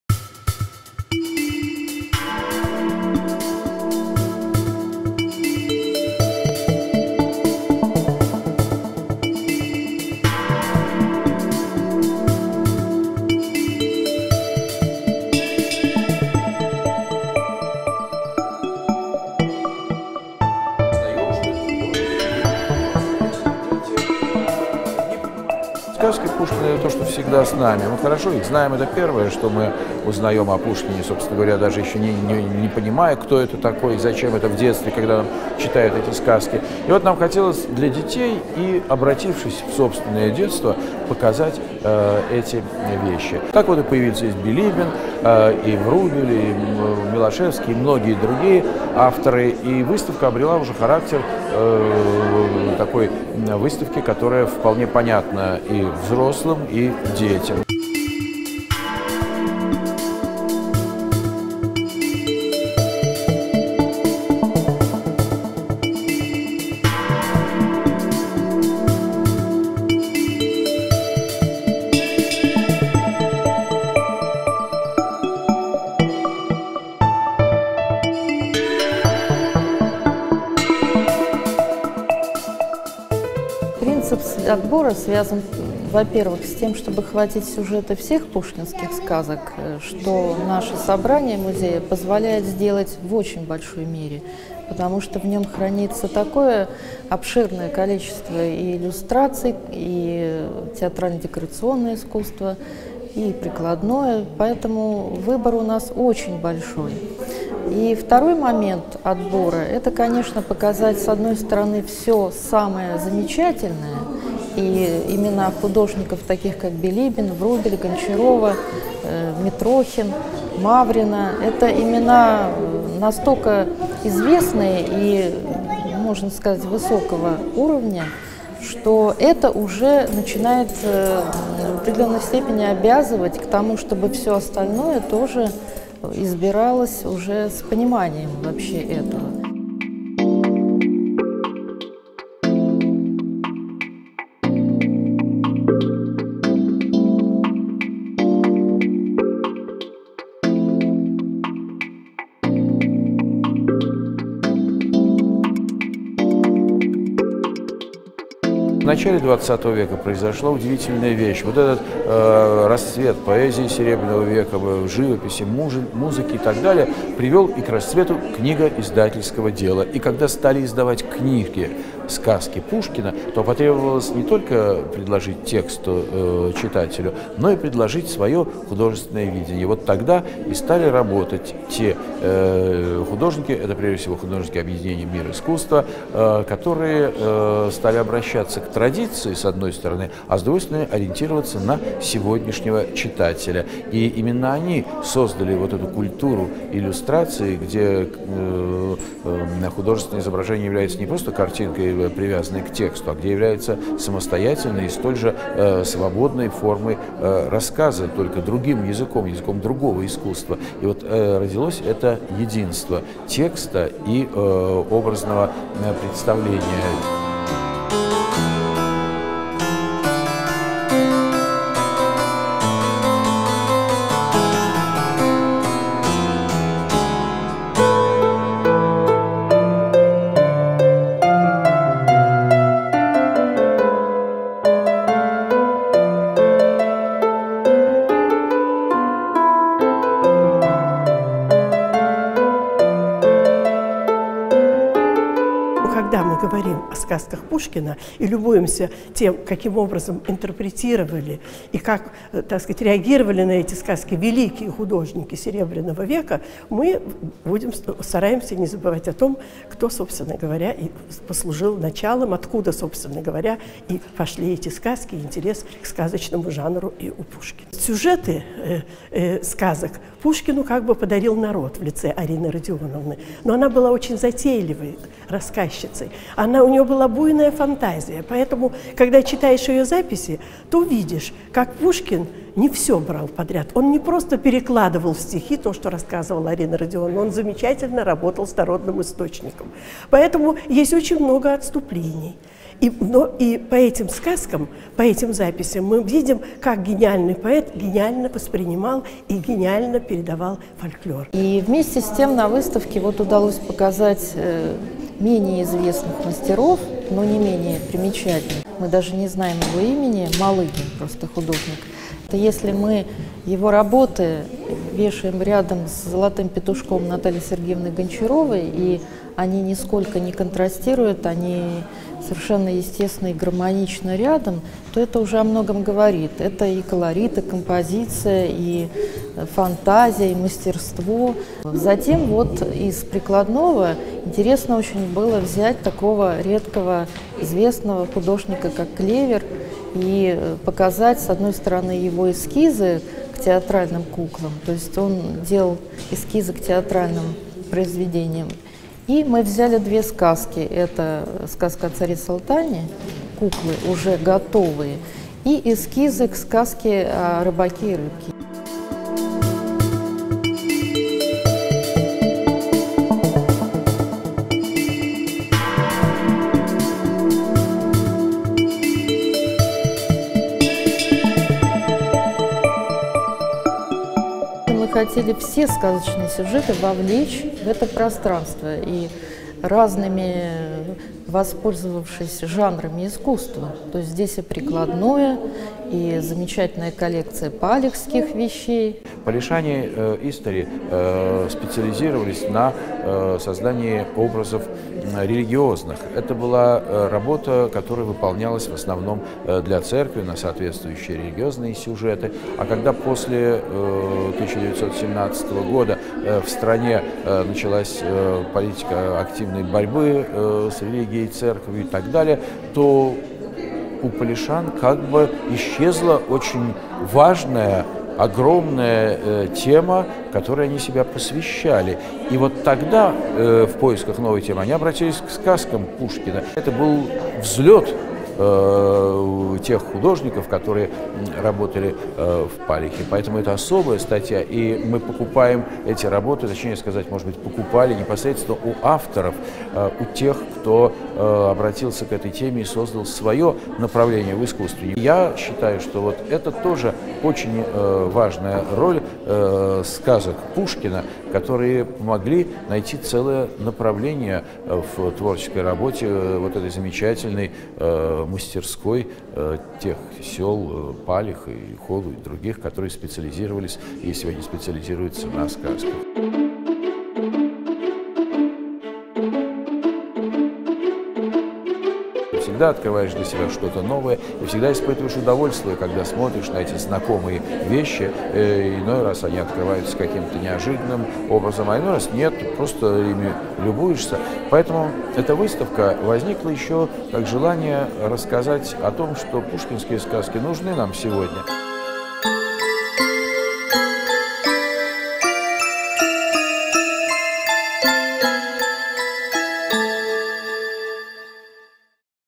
Выставка "Сказки Пушкина", Всероссийский музей А.С. Пушкина. Санкт-Петербург, 2011
В сюжете участвуют: Сергей Некрасов, директор Всероййского музея А. С. Пушкина; Раиса Иезуитова, заместитель директора Всероссийского музея А. С. Пушкина, Алла Рабинянц, куратор выставки.

В экспозиции представлены работы Билибина, Врубеля, Гончаровой, преимущественно художников "Мира искусства", а также художники Палеха и собственно культура иллюстрации пушкинских сказок, соотнесение текста и его образного представления.